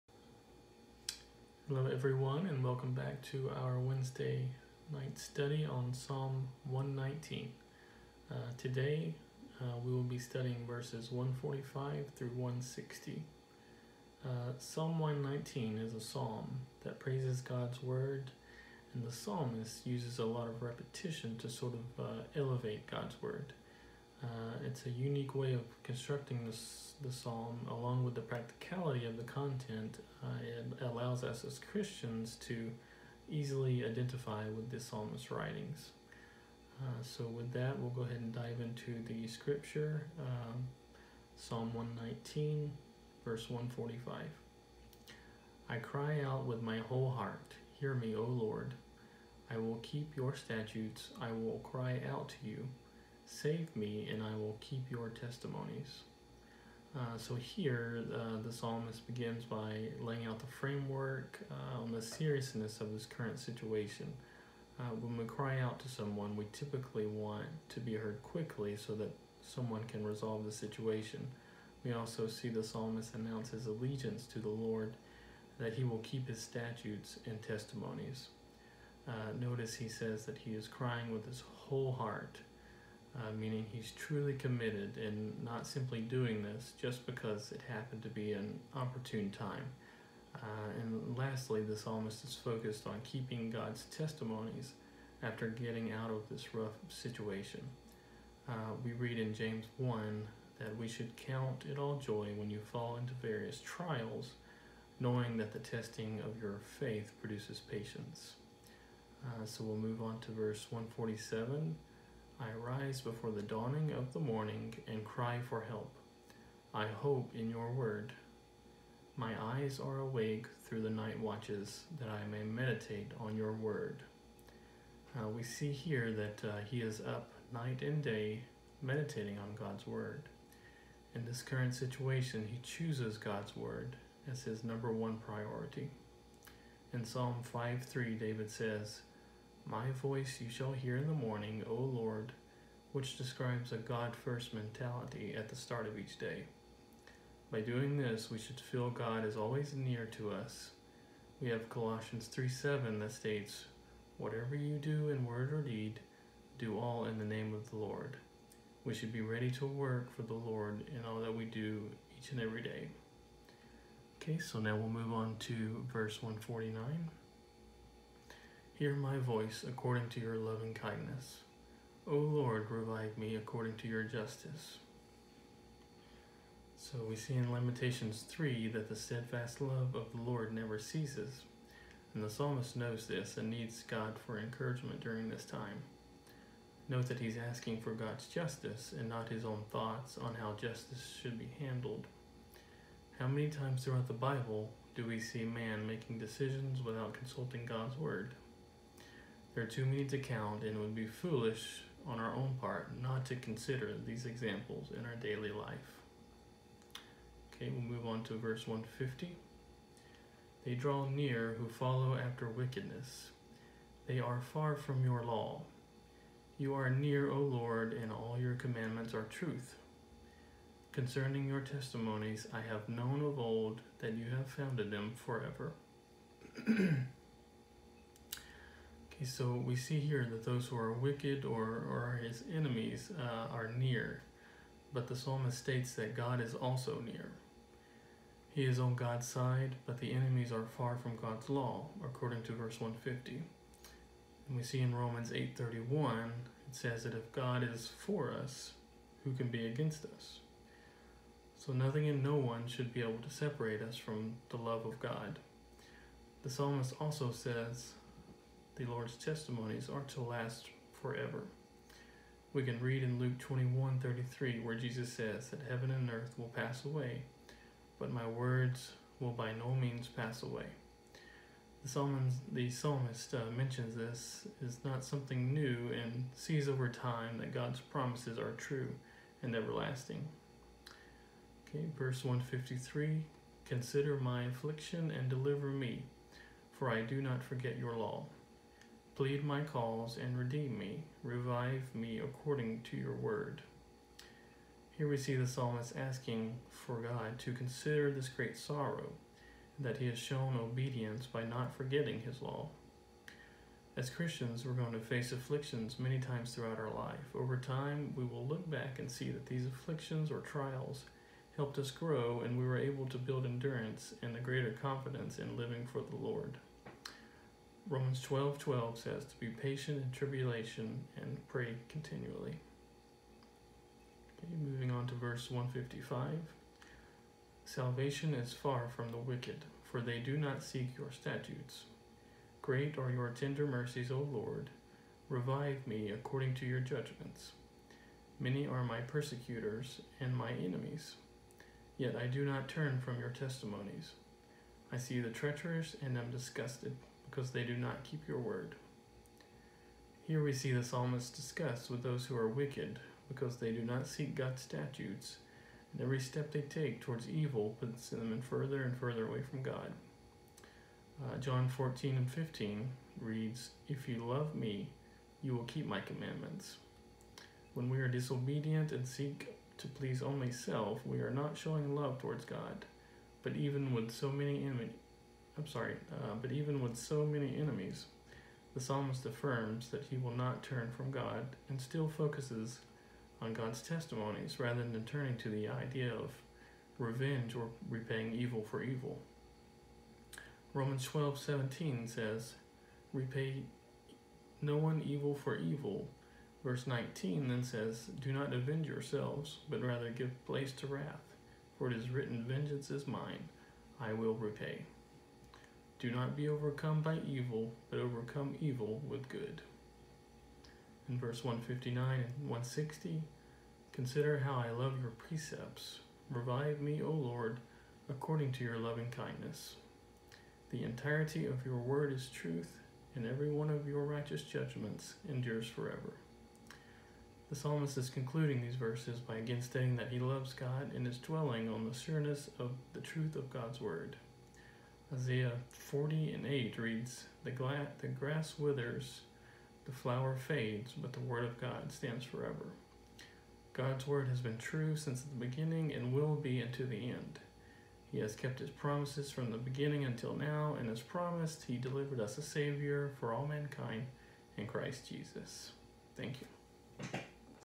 Psalm of the Word Lesson 11
The longest Psalm and the longest chapter in the Bible deserves our utmost efforts at applying this Psalm of the Word: Psalm 119. Lesson 11 | 119:145-160. Presented by Nathan Shows.